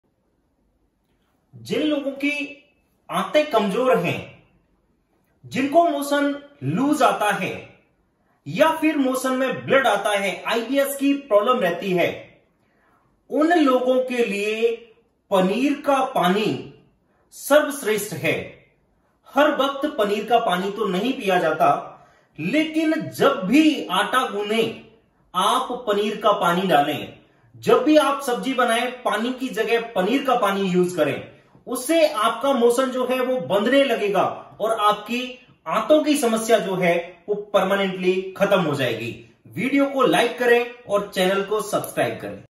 IBS की समस्या होने पर करें यह शानदार उपाय।DevendraSharmaNeurotherapy।
नमस्कार 🙏
ये विडियो ऐसे लोगों के लिए बहुत ज़बरदस्त है जिनको बार-बार व लूज़ मोशन आता है
ऐसे लोगों की समस्या को ही IBS (irritable bowel syndrome) की बीमारी कहते है
ये उपाय उनकी समस्या में बहुत लाभदायक सिद्ध हो रहा है

आप हमारे साथ बने रहे